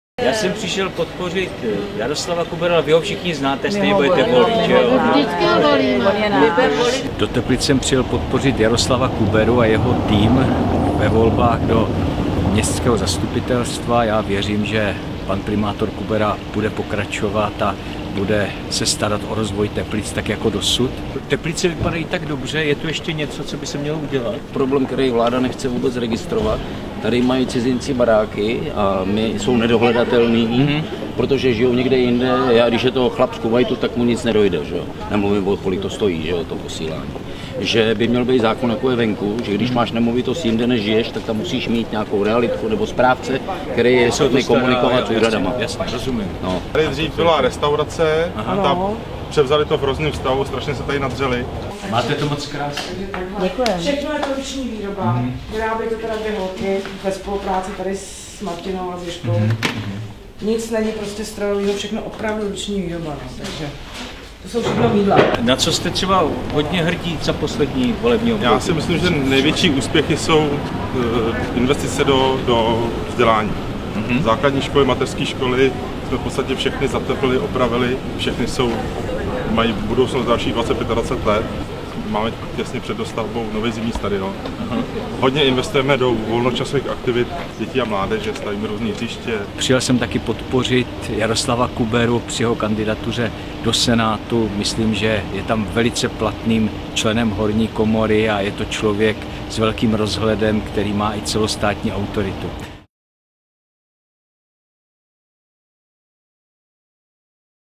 V ulicích Teplic to vypadalo, že se Jaroslav Kubera chystá zvítězit již v 1. kole
Jaroslav Kubera netřeba představovat. A v Teplicích už vůbec ne. Dnes to podle reakcí občanů v ulicích vypadalo, že se chystá zvítězit již v 1. kole a teplická ODS získá v komunálních volbách nejméně 50 %. Moc bych to jemu i Teplicím přál.